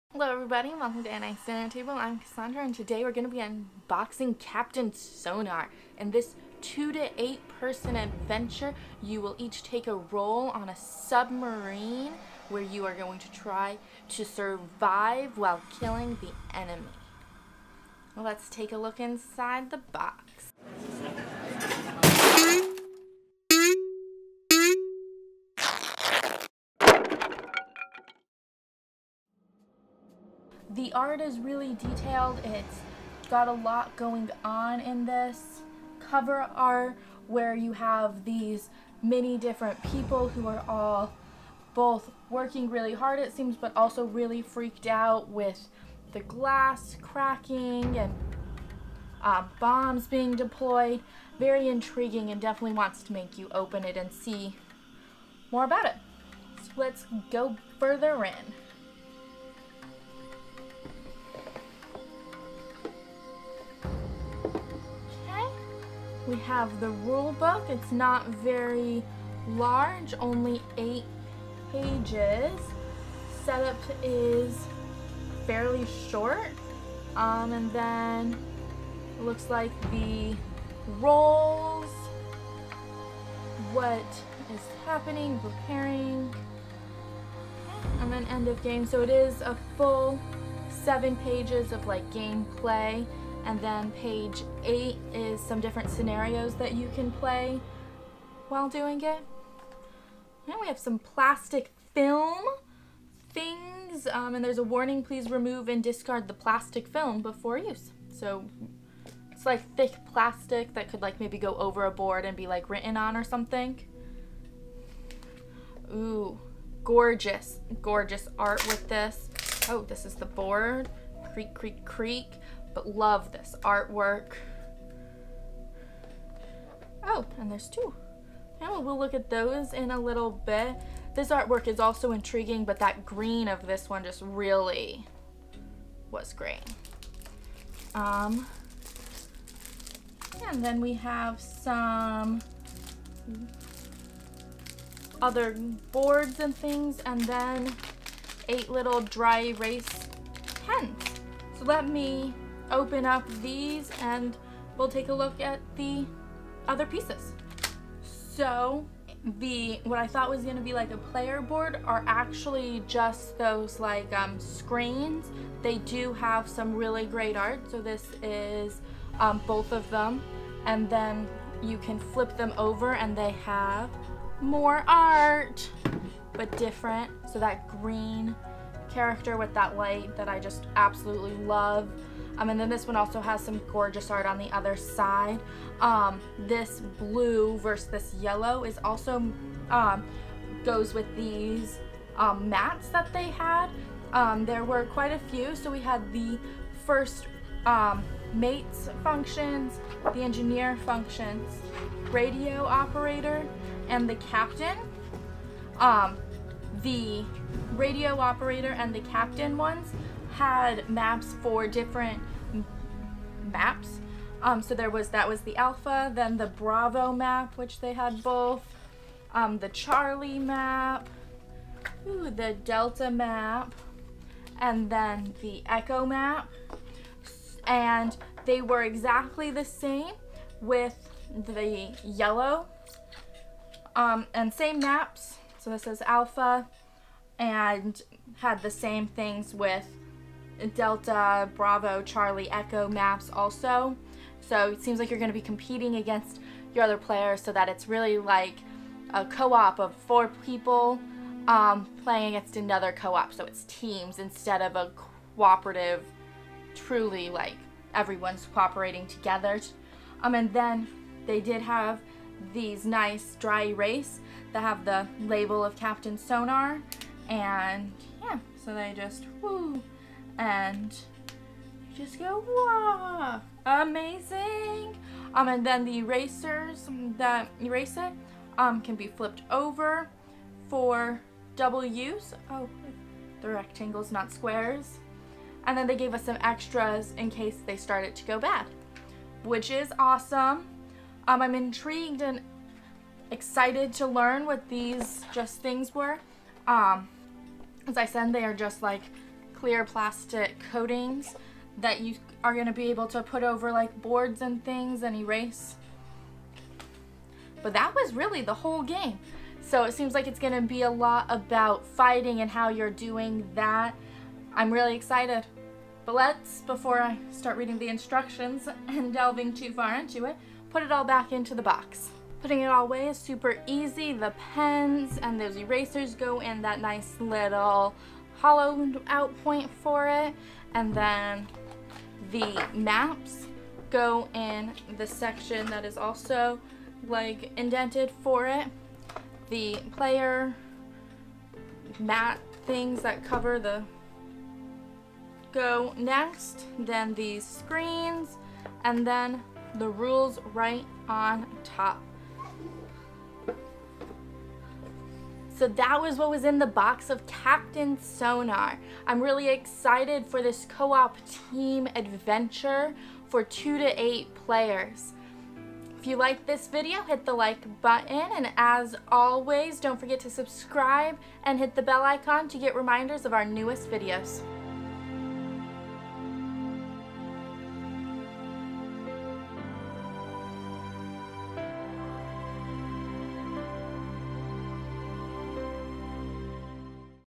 Captain Sonar Unboxing
See what is inside the box of Captain Sonar. In this game you will split into two teams, each person takes on the roles of submarine works as you battle it out underwater.